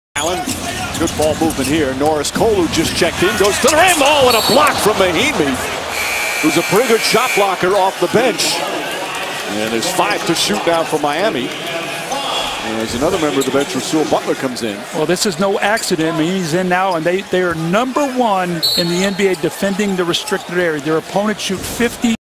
Ian Mahinmi Monster Block on Norris Cole
Miami Heat  Vs Indiana Pacers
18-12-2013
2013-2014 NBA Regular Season